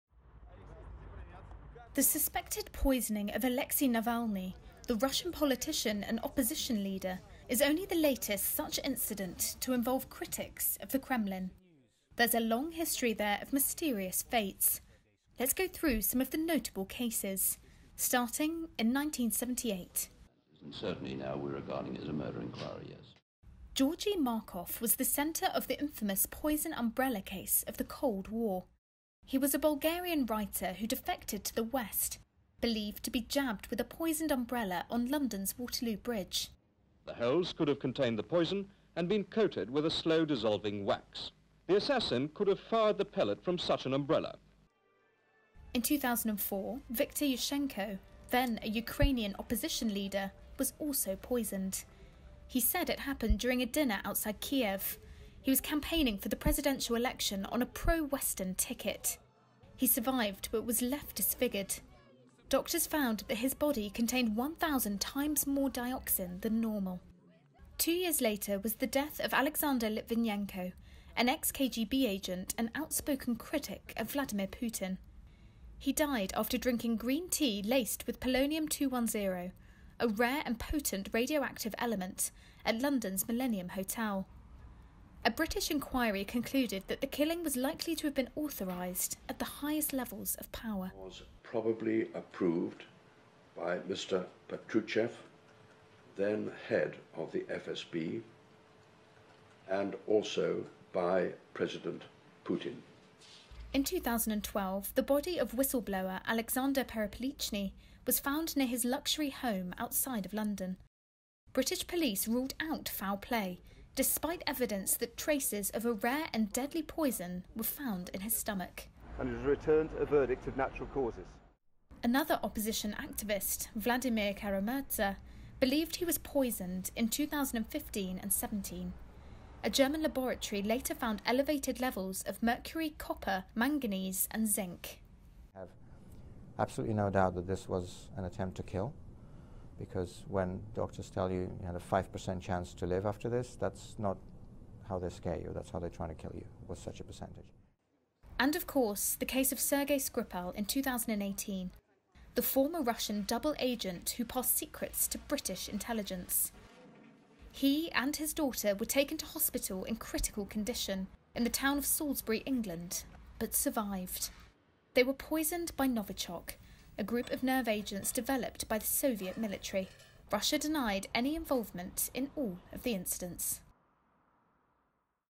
Kremlin foes who have suffered mysterious fates
The suspected poisoning of opposition leader Alexei Navalny is only the latest of its kind to involve critics of the Kremlin. Here's a look at the long history of mysterious fates.